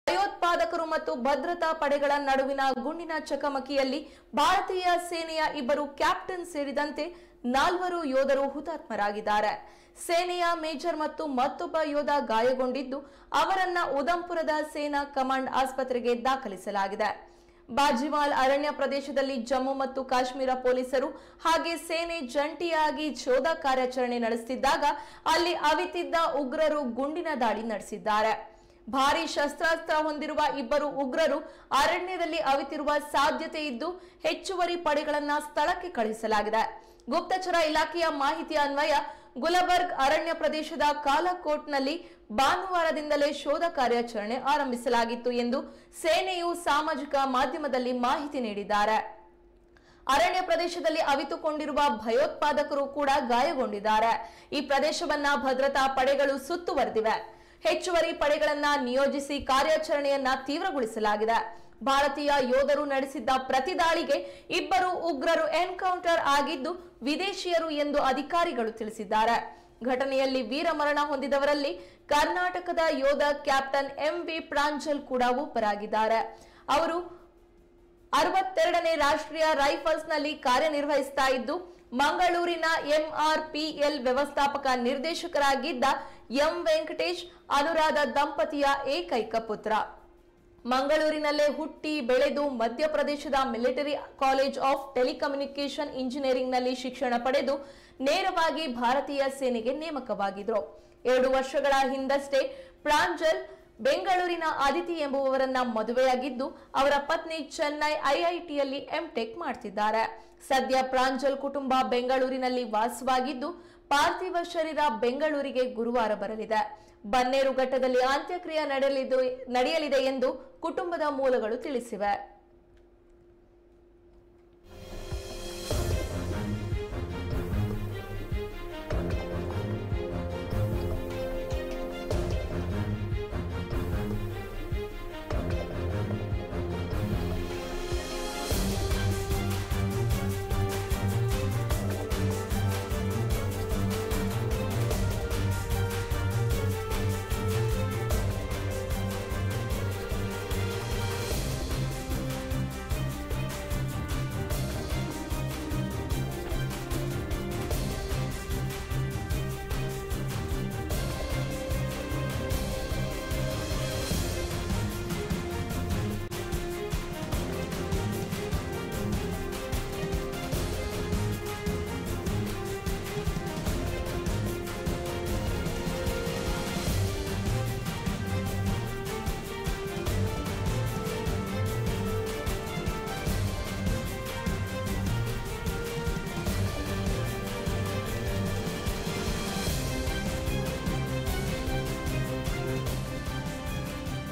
ಕರ್ನಾಟಕದ ಕ್ಯಾಪ್ಟನ್ ಹುತಾತ್ಮ, ಕಾಶ್ಮಿರದಲ್ಲಿ ಉಗ್ರರ ಜೊತೆಗೆ ಕಾದಾಟ ನಾಲ್ವರು ಯೋಧರ ವೀರಮರಣ
ಜಮ್ಮು ಮತ್ತು ಕಾಶ್ಮೀರದ ರಜೌರಿ ಜಿಲ್ಲೆಯ ಗಡಿಪ್ರದೇಶದಲ್ಲಿ ಭಯೋತ್ಪಾದಕರು ಮತ್ತು ಭದ್ರತಾ ಪಡೆಗಳ ನಡುವಿನ ಗುಂಡಿನ ಚಕಮಕಿಯಲ್ಲಿ ಭಾರತೀಂii ಸೇನೆಯ ಇಬ್ಬರು ಕ್ಯಾಪ್ಟನ್ ಸೇರಿದಂತೆ ನಾಲ್ವರು ಯೋಧರು ಹುತಾತ್ಮರಾಗಿದ್ದಾರೆ. ಸೇನೆಯ ಮೇಜರ್ ಹಾಗೂ ಮತ್ತೊಬ್ಬ ಯೋಧ ಗಾಯಗೊಂಡಿದ್ದು ಅವರನ್ನು ಉಧಂಪುರದ ಸೇನಾ ಕಮಾಂಡ್ ಆಸ್ಪತ್ರೆಗೆ ದಾಖಲಿಸಲಾಗಿದೆ. ಬಾಜಿಮಾಲ್ ಅರಣ್ಯ ಪ್ರದೇಶದಲ್ಲಿ ಜಮ್ಮು ಮತ್ತು ಕಾಶ್ಮೀರ ಪೋಲಿಸರು ಹಾಗೂ ಸೇನೆ ಜಂಟಿಯಾಗಿ ಶೋಧ ಕಾರ್ಯಚರಣೆ ನಡೆಸುತ್ತಿದ್ದಾಗ ಅಲ್ಲಿ ಅವಿತಿದ್ದ ಉಗ್ರರು ಗುಂಡಿನ ದಾಳಿ ನಡೆಸಿದ್ದಾರೆ. ಭಾರಿ ಶಸ್ತ್ರಾಸ್ತ್ರ ಹೊದಿರುವ ಇಬ್ಬರು ಉಗ್ರರು ಅರಣ್ಯದಲ್ಲಿ ಅವಿತಿರುವ ಸಾಧ್ಯತೆ ಇದ್ದು ಹೆಚ್ಚುವರಿ ಪಡೆಗಳನ್ನು ಸ್ಥಳಕ್ಕೆ ಕಳುಹಿಸಲಾಗಿದೆ. ಗುಪ್ತಚರ ಇಲಾಖೆಯ ಮಾಹಿತಿ ಅನ್ವಯ ಗುಲಬರ್ಗ್ ಅರಣ್ಯ ಪ್ರದೇಶದ ಕಾಲಕೋಟ್‌ನಲ್ಲಿ ಭಾನುವಾರದಿಂದಲೇ ಶೋಧ ಕಾರ್ಯಚರಣೆ ಆರಂಭಿಸಲಾಗಿತ್ತು ಎಂದು ಸೇನೆಯು ಸಮಾಜಿಕ ಮಾಧ್ಯಮದಲ್ಲಿ ಮಾಹಿತಿ ನೀಡಿದಾರೆ. ಆರಣ್ಯಪ್ರದೇಶದಲ್ಲಿ ಅವಿತುಕೊಂಡಿರುವ ಭಯೋತ್ಪಾದಕರು ಕೂಡ ಗಾಯಗೊಂಡಿದ್ದಾರೆ. ಈ ಪ್ರದೇಶವನ್ನು ಭದ್ರತಾ ಪಡೆಗಳು ಸುತ್ತುವರಿದಿವೆ. ಹೆಚ್ಚುವರಿ ಪಡೆಗಳನ್ನು ನಿಯೋಜಿಸಿ ಕಾರ್ಯಚರಣೆಯನ್ನು ತೀವ್ರಗೋಳಿಸಲಾಗಿದೆ. ಭಾರತಿಯ ಯೋಧರು ನಡೆಸಿದ್ದ ಪ್ರತಿ ದಾಳಿಗೆ ಇಬ್ಬರು ಉಗ್ರರು ಎನ್‌ಕೌಂಡರ್ ಅಗಿದ್ದು ವಿದೇಶಿಯರು ಎಂದು ಅಧಿಕಾರಿಗಳು ತಿಳಿಸಿದ್ದಾರೆ. ಘಟನೆಯಲ್ಲಿ ವೀರ ಮರಣ ಹೊಂದಿದವರಲ್ಲಿ ಕರ್ನಾಟಕದ ಯೋಧ ಕ್ಯಾಪ್ಟನ್ ಎಂ.ವಿ.ಪ್ರಾಂಜಲ್ ಕೂಡ ಒಬ್ಬರಾಗಿದ್ದಾರೆ. ಅವರು ೬೩ನೇ ರಾಷ್ಟ್ರೀಯ ರೈಫಲ್ಸ್‌ನಲ್ಲಿ ಕಾರ್ಯನಿರ್ವಹಿಸುತ್ತಿದ್ದು ಮಂಗಳೂರಿನ ಎಂಆರ್‌ಪಿಎಲ್ ವ್ಯವಸ್ಥಾಪಕ ನಿರ್ದೇಶಕರಾಗಿದ್ದ ಎಂ.ವೆಂಕಟೆಶ್, ಅನುರಾಧ ದಂಪತಿಯ ಏಕೈಕ ಪುತ್ರ. ಮಂಗಳೂರಿನಲ್ಲೇ ಹುಟ್ಟಿ ಬೆಳೆದು ಮಧ್ಯಪ್ರದೇಶದ ಮಿಲಿಟರಿ ಕಾಲೇಜ್ ಆಫ್ ಟೆಲಿಕಮ್ಯುನಿಕೇಶನ್ ಇಂಜಿನಿಯರಿಂಗ್‌ನಲ್ಲಿ ಶಿಕ್ಷಣ ಪಡೆದು ನೇರವಾಗಿ ಭಾರತೀಯ ಸೇನೆಗೆ ನೇಮಕವಾಗಿದ್ದರು. ಎರಡು ವರ್ಷಗಳ ಹಿಂದಷ್ಟೇ ಪ್ರಾಂಜಲ್ ಬೆಂಗಳೂರಿನ ಅದಿತಿ ಎಂಬುವರನ್ನು ಮದುವೆಯಾಗಿದ್ದು ಅವರ ಪತ್ನಿ ಚೆನ್ನೈ ಐಐಟಿಯಲ್ಲಿ ಎಂ.ಟೆಕ್ ಮಾಡುತ್ತಿದ್ದಾರೆ. ಸದ್ಯ ಪ್ರಾಂಜಲ್ ಕುಟುಂಬ ಬೆಂಗಳೂರಿನಲ್ಲಿ ವಾಸವಾಗಿದ್ದು ಪಾರ್ಥಿವ ಶರೀರ ಬೆಂಗಳೂರಿಗೆ ಗುರುವಾರ ಬರಲಿದೆ. ಬನ್ನೇರುಘಟ್ಟದಲ್ಲಿ ಅಂತ್ಯಕ್ರಿಯೆ ನಡೆಯಲಿದೆ ಎಂದು ಕುಟುಂಬ ಮೂಲಗಳು ತಿಳಿಸಿವೆ.